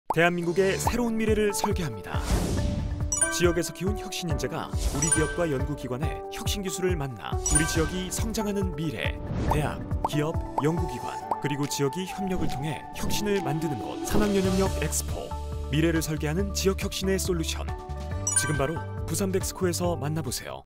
2024 산학연협력 EXPO 홍보영상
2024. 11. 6. (수) ~ 11. 8. (금)

“미래를 설계하는 지역혁신의 솔루션”
2024 산학연협력 EXPO가 부산 BEXCO에서 개최됩니다.

교육부, 과학기술정보통신부, 부산광역시가 공동으로 주최하고,
한국연구재단이 주관하는 이번 행사에 여러분들의 많은 참여 부탁드립니다✨